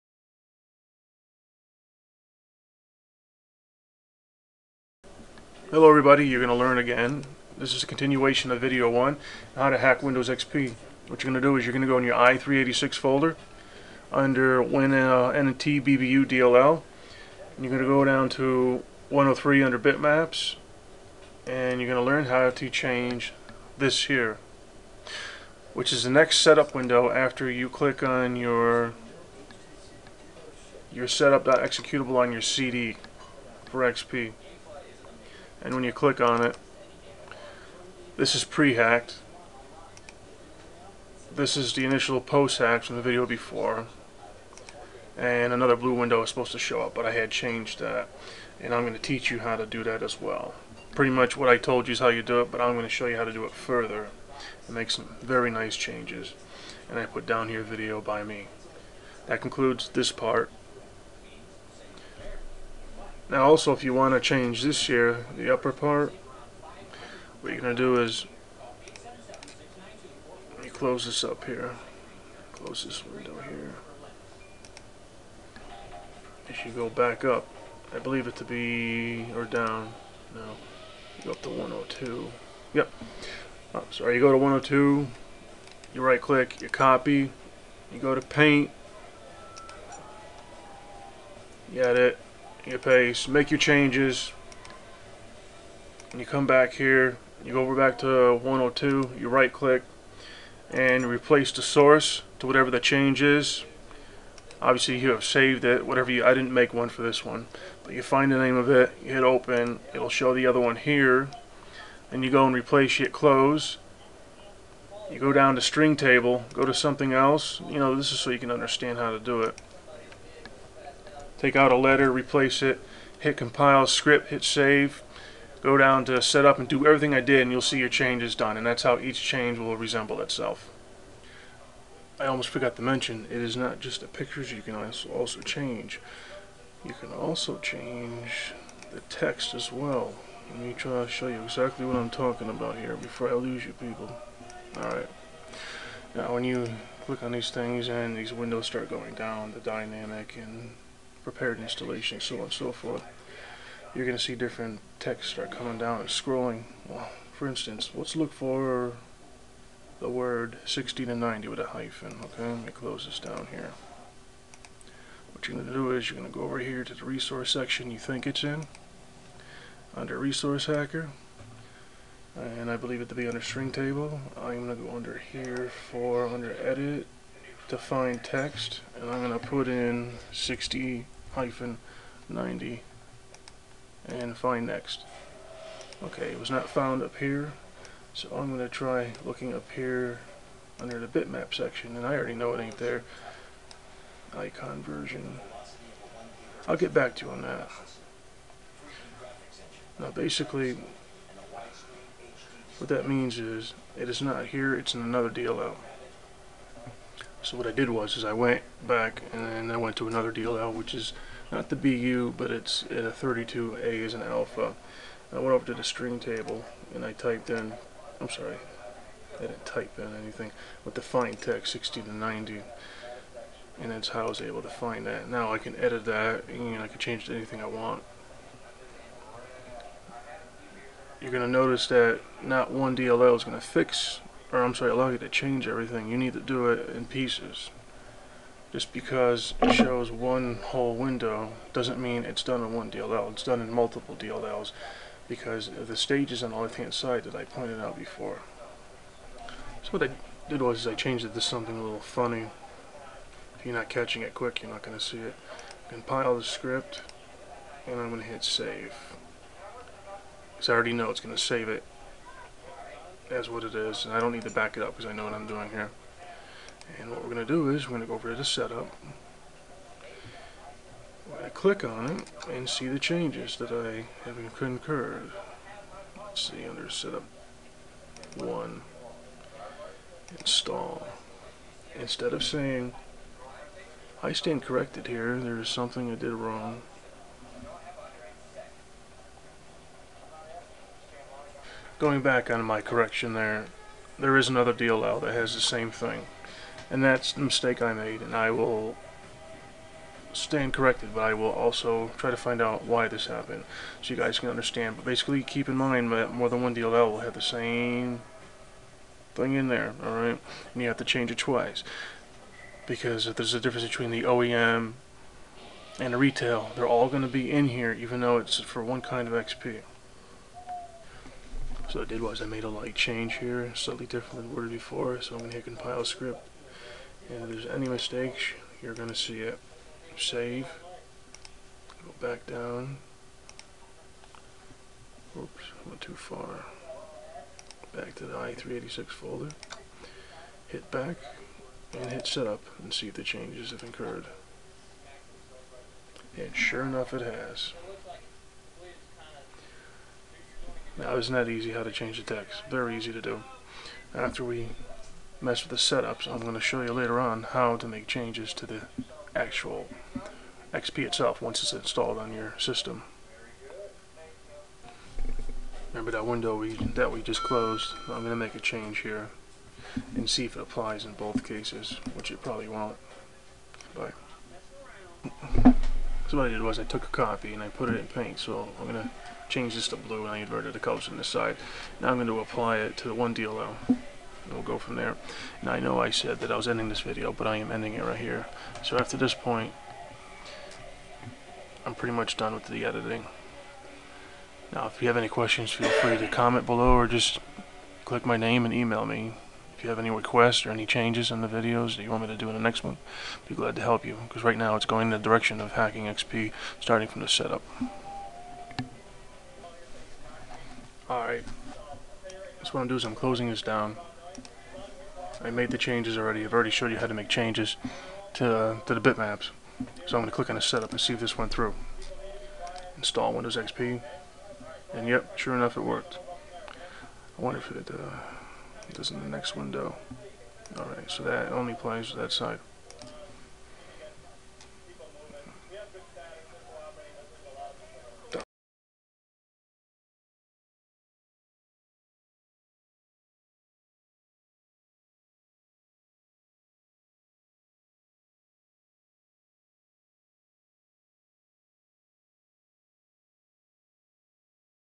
Part 2: Learn How to Hack Windows XP CD Setup & Make a Custom Windows XP .ISO 2
This is Video #2 for Changing/Hacking your Windows XP Setup , Startup. This is step 1 of many to complete the Teaching of XP .ISO Hacking. Which will allow you to change most aspects including installation's Fonts , Colors , Properties , Text & so on. Even with nLite your scope to customize your XP ISO is Limited.
You'll need ResHack:
**Even if You Have Resource Hacker , you'll Need THIS Version over yours.